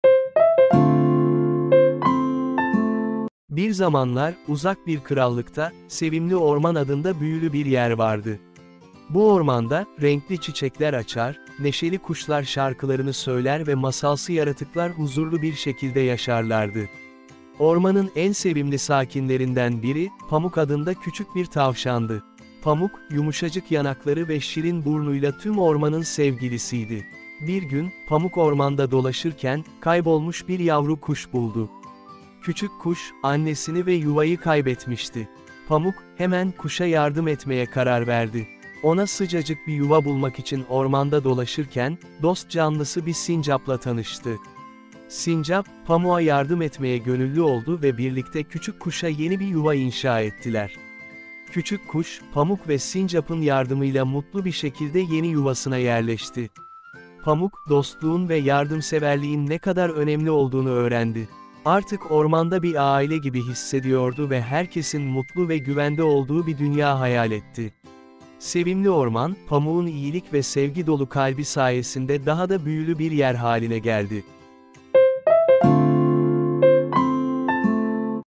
🌳 Pamuk'un Yardımseverliği: Sevimli Ormanın Masalsı Hikayesi 🐰
🐇Sevimli Orman'ın büyülü atmosferine adım atın ve Pamuk'un dokunaklı hikayesini keşfedin! 🌳Bu masalsı hikaye, dostluğun, yardımseverliğin ve bir araya gelmenin gücünü anlatıyor. Sizleri, iyilik dolu bir dünyanın mümkünlüğüne inanmaya davet ediyoruz.🌿